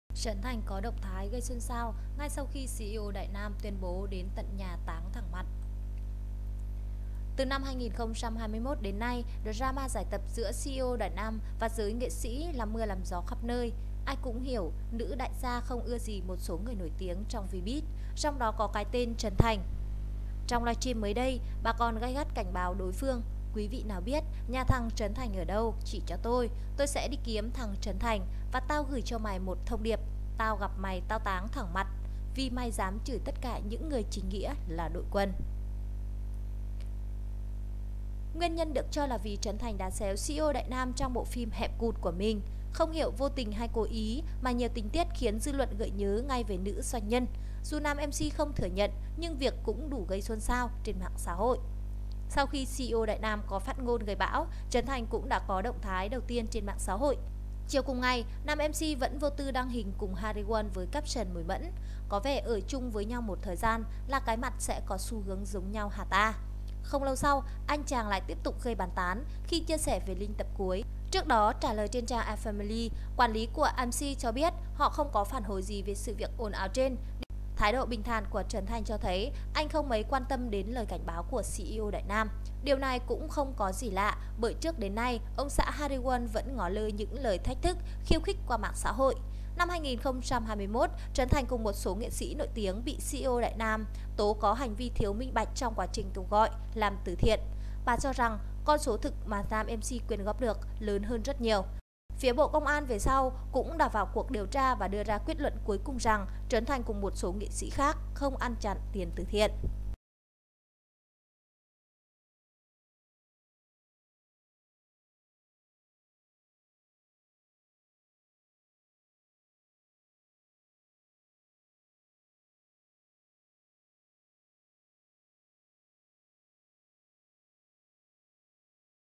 Trấn Thành TUYÊN BỐ ĐANH THÉP ngay sau khi CEO Đại Nam tuyên bố đến tận nhà ‘TÁ.NG THẲNG MẶT'.
Trấn Thành TUYÊN BỐ ĐANH THÉP gây xôn xao ngay sau khi CEO Đại Nam tuyên bố đến tận nhà ‘táng thẳng mặt'.